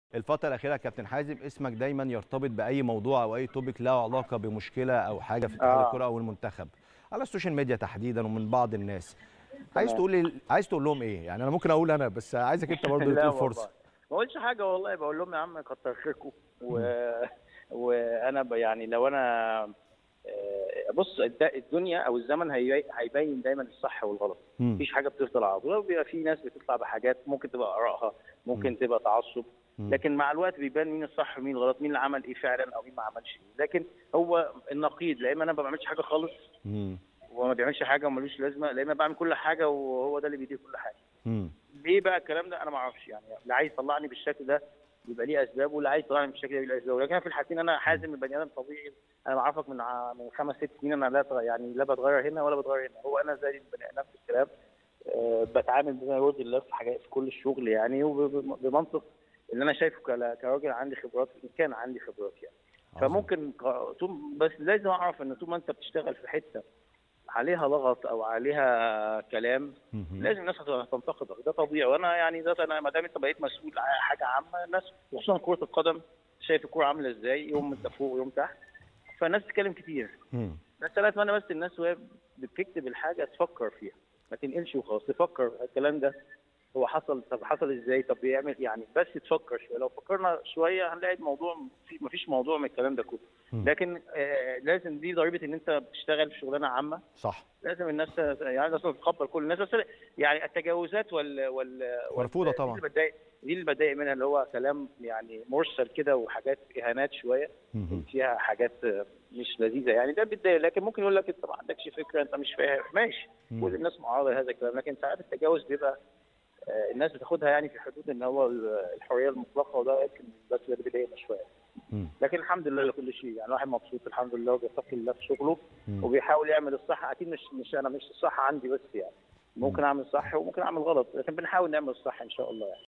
💣🔥 "بعد الهجوم عليه الفترة الأخيرة" .. حازم امام: طول ما انت بتشتغل لازم الناس تنتقدك
رقم 10 | "بعد الهجوم عليه الفترة الأخيرة" .. حازم امام: طول ما انت بتشتغل لازم الناس تنتقدك .. وبتعامل بما يرضي الله في شغلي ومع الوقت هيبان مين الصح ومين الغلط 💣🔥